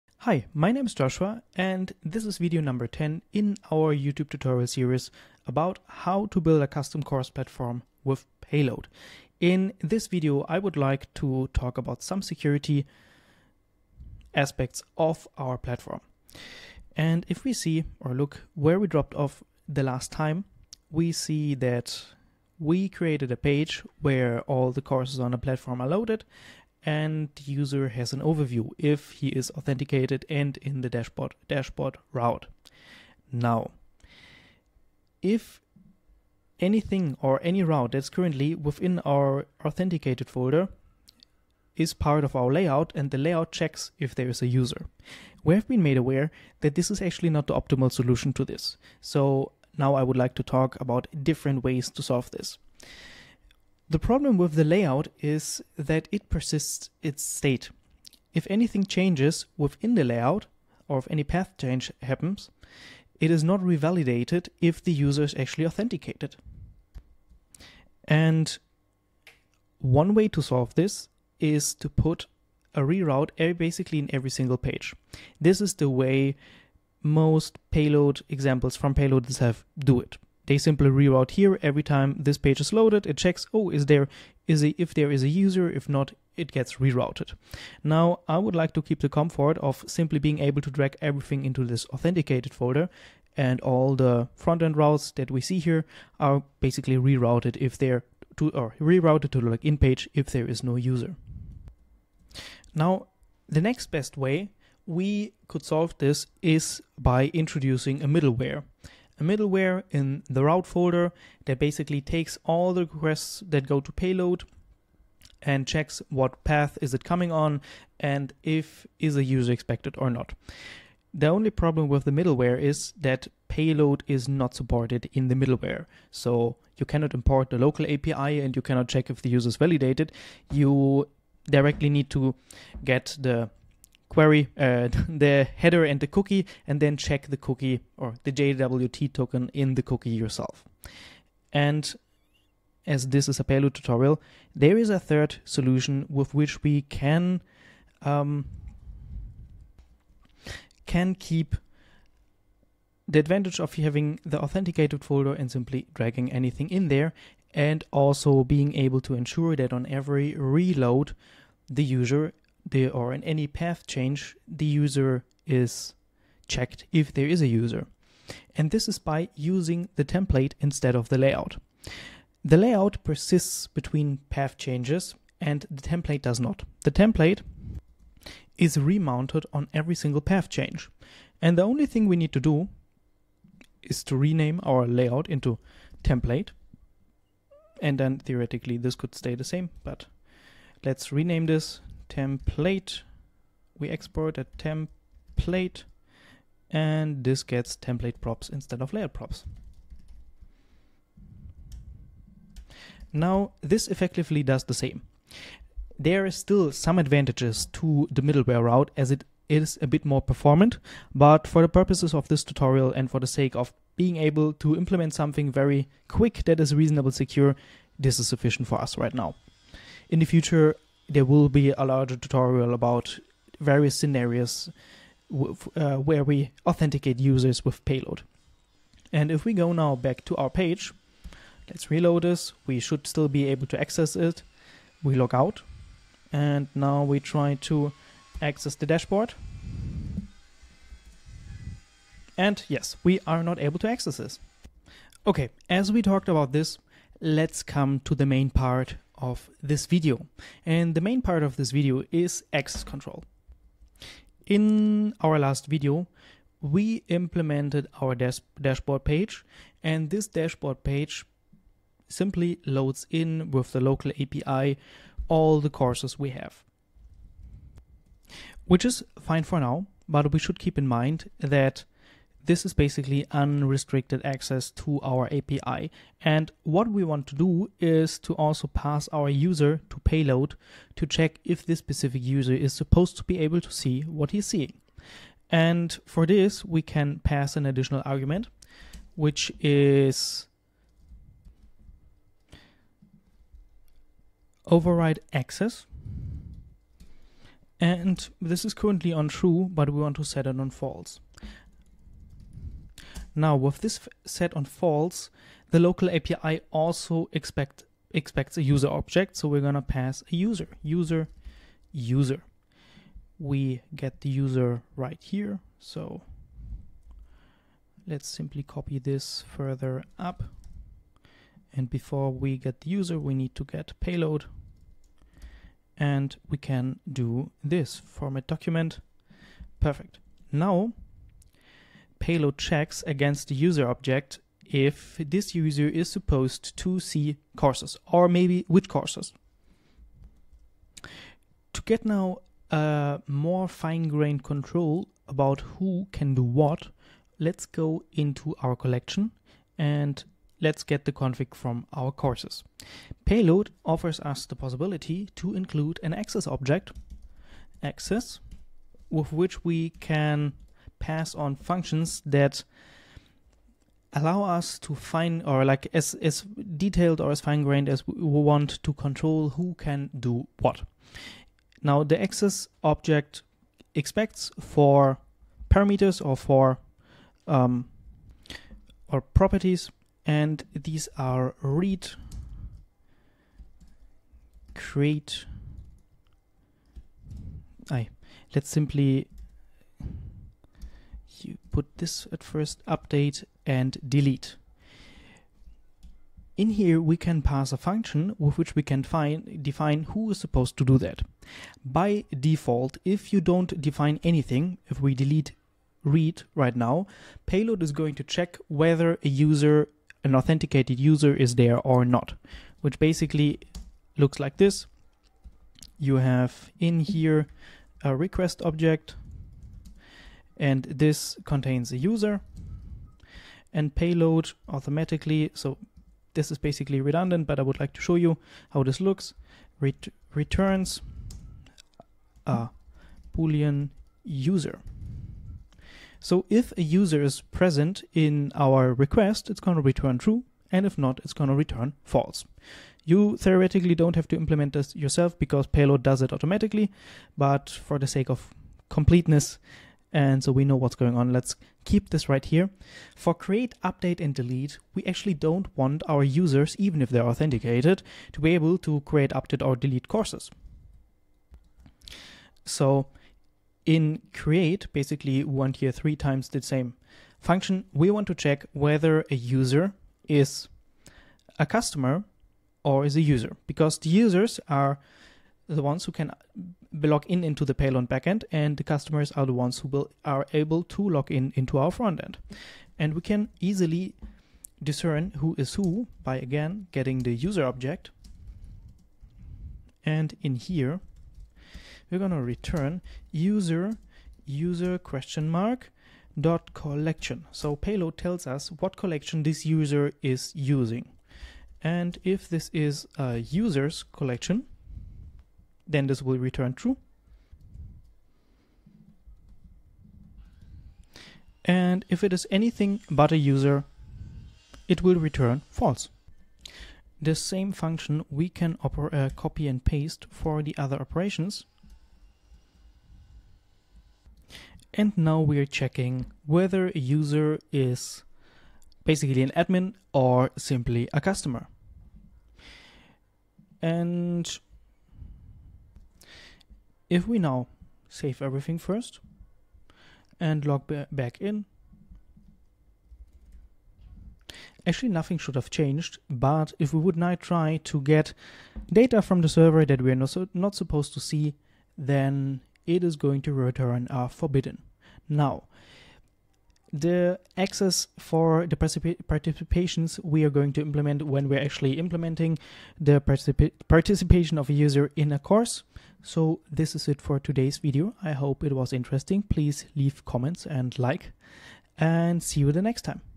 #10 - Access rights - Security and Local API - Course platform with Payload 3.0, Next.js 15 and TS
🎥 Welcome to Video 10 of our Payload Tutorial Series! 🚀

In this video, we focus on improving the security of our custom course platform built with Payload CMS. We discuss layout authentication issues, introduce alternative solutions using templates, and dive deep into implementing access control for our API and collections.

📌 Chapters:
00:00 Introduction
00:09 Reviewing the Layout Authentication Problem
01:06 Rerouting in Pages vs. Using Middleware
02:21 Using Templates for Improved Security
04:13 Implementing Access Control in Payload
05:53 Setting Up Access Rules for Collections
07:06 Handling User Roles in Create, Update, and Delete Operations
08:45 Finalizing and Testing Access Restrictions
09:26 Closing Remarks and Next Steps

This video is part of the Series: Building a course membership platform with Payload 3.0, Next.js 15 and TypeScript

Have questions or feedback? Drop them in the comments below. Your input helps us tailor content to your needs! ❤️

🌟 Next Video Preview:
In the next video, we’ll explore user participation tracking in courses and build a dashboard for enhanced interactivity.